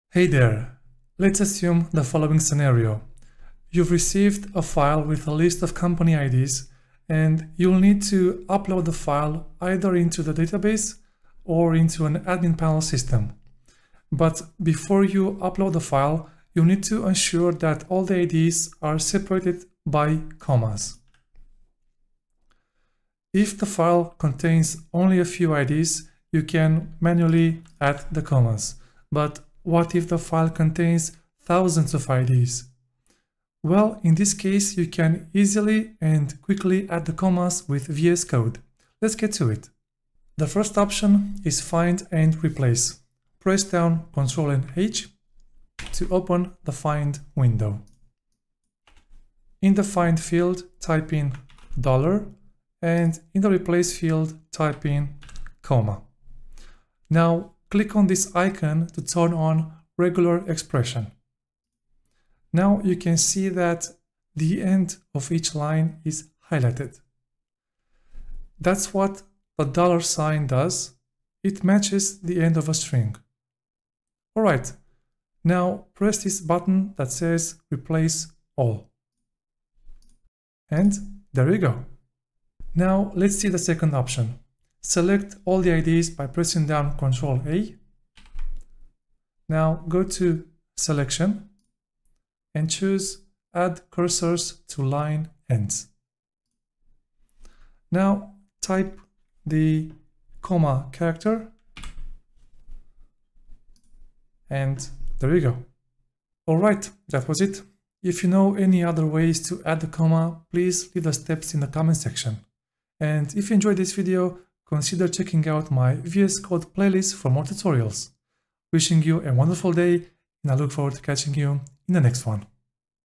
VS Code: Add Comma to Each Line
In this video, you'll learn how to easily and quickly add commas to a list of IDs.